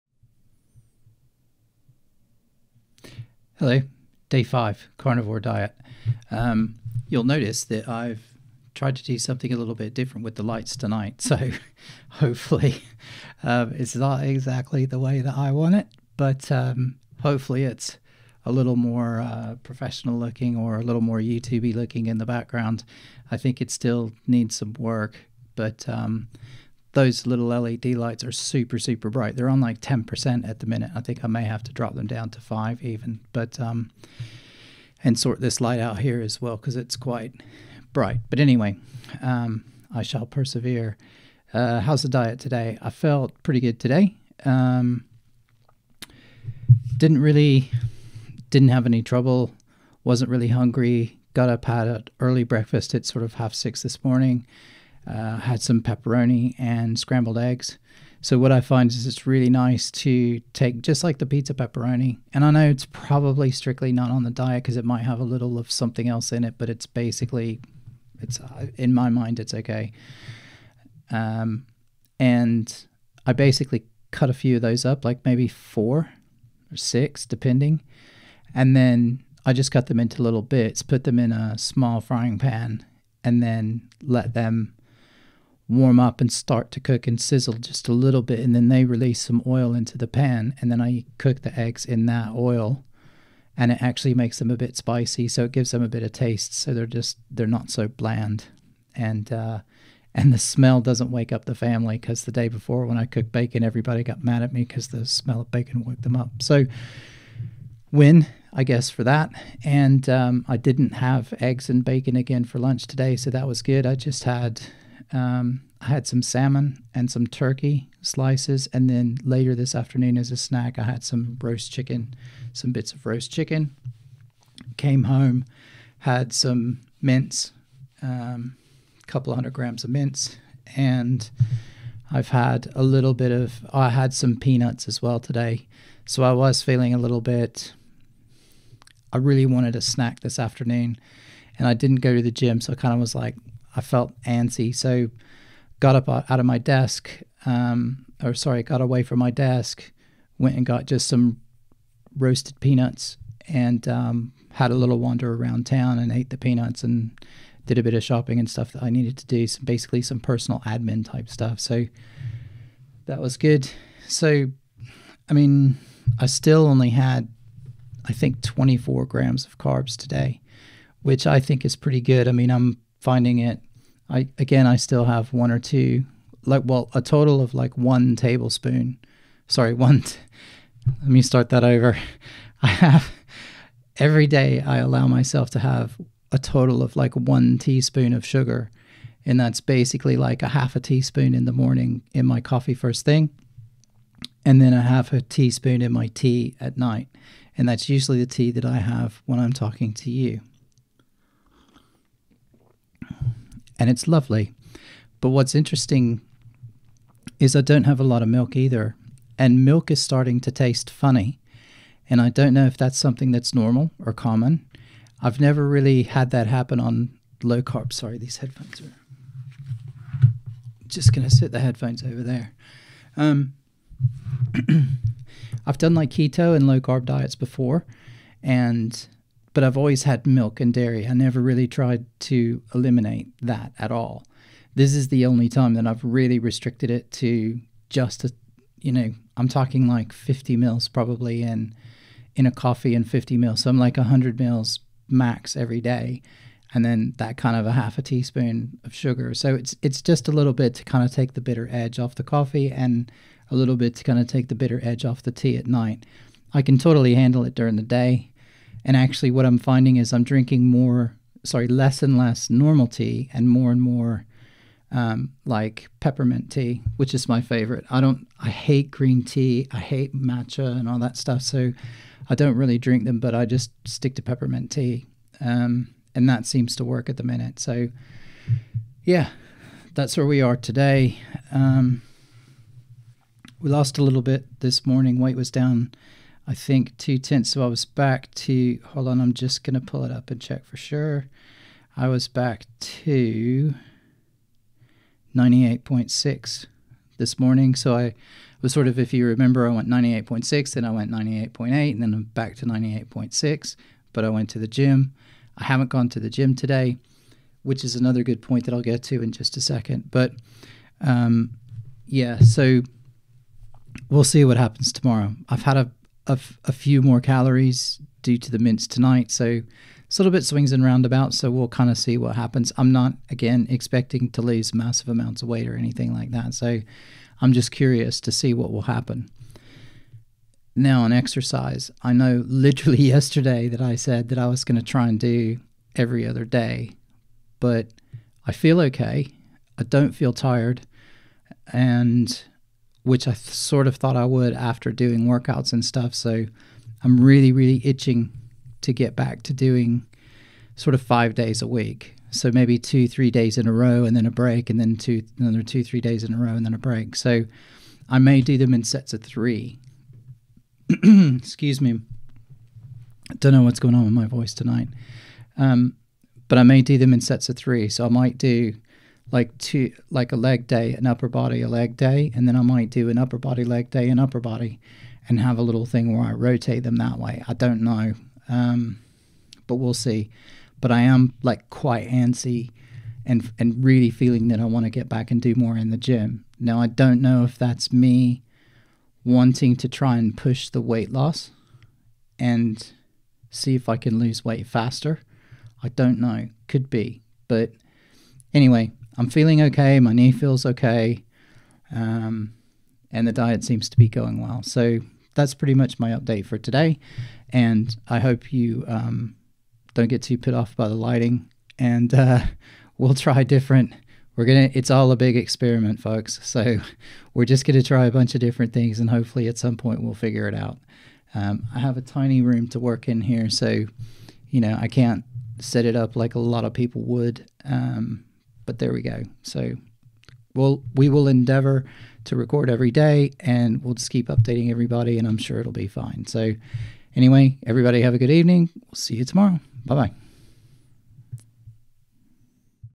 Carnivore Diet Day 5 - Got milk?
Milk is starting to taste funny and I want to get back in the gym more often.